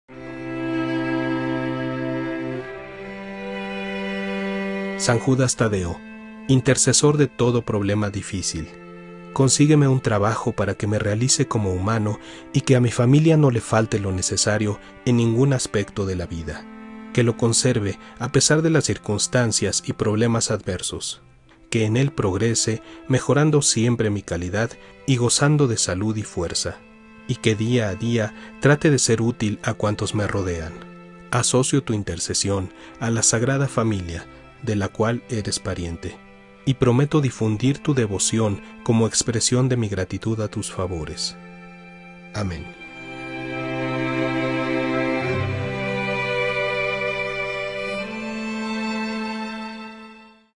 ORACIÓN A SAN JUDAS TADEO PARA CONSEGUIR TRABAJO (MUY MILAGROSA!!) | Fe y Salvación
San Judas Tadeo, intercesor de todo problema difícil: consígueme un trabajo en que me realice como humano y que a mi familia no le falte lo necesario en ningún aspecto de la vida. Que lo conserve a pesar de las circunstancias y problemas adversos. Que en él progrese mejorando siempre mi calidad y gozando de salud y fuerza. Y que día a día trate de ser útil a cuantos me rodean.
Asocio tu intercesión a la Sagrada Familia, de la cual eres pariente y prometo difundir tu devoción como expresión de mi gratitud a tus favores.
Amén.

(Rezarla todos los días hasta conseguir empleo y después dar gracias difundiendo el mensaje)